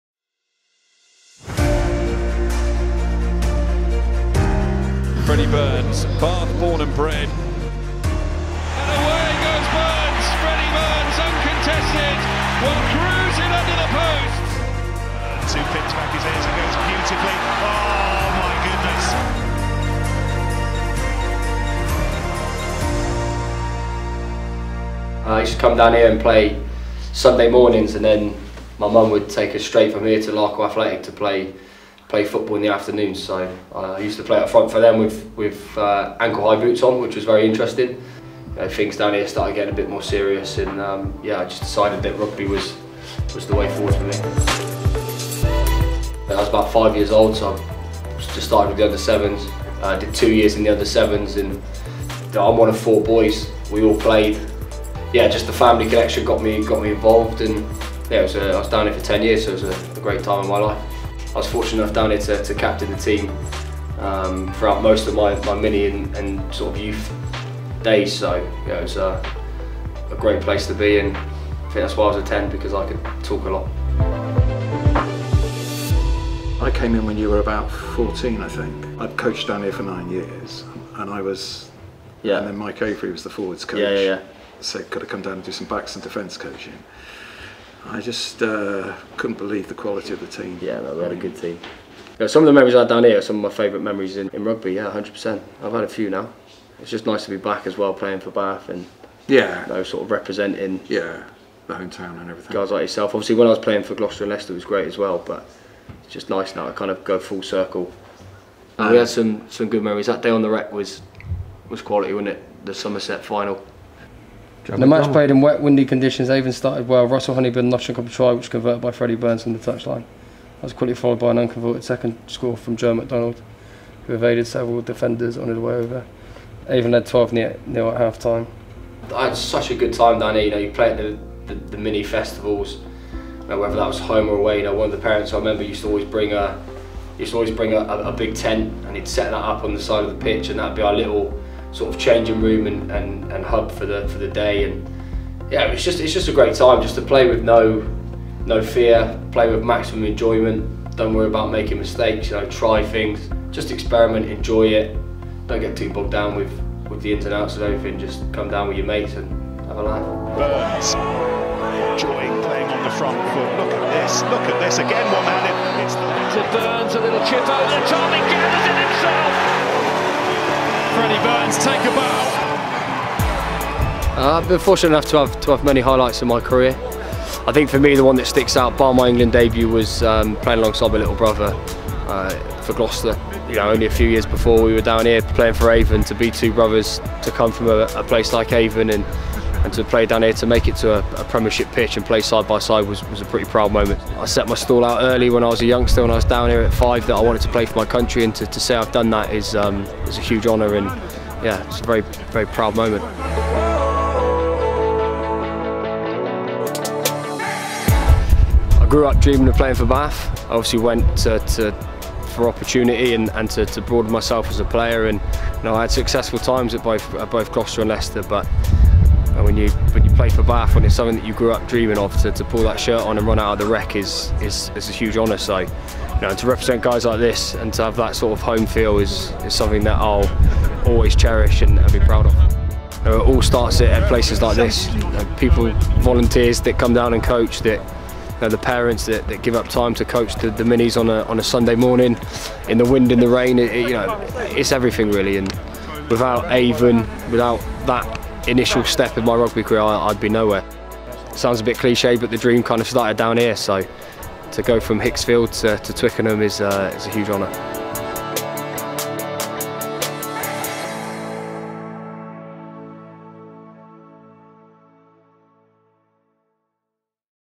Grassroots Episode One | Freddie Burns returns to Avon RFC
Over the course of the 2019/20 season, Bath Rugby's Community team will be taking players back to their Grassroots Clubs to delve into stories of their rugby journey and how they got involved in the game.

Bath Rugby would like to recognise the role local grassroots clubs have played in creating our professional athletes and championing the work volunteers carry out across our catchment area.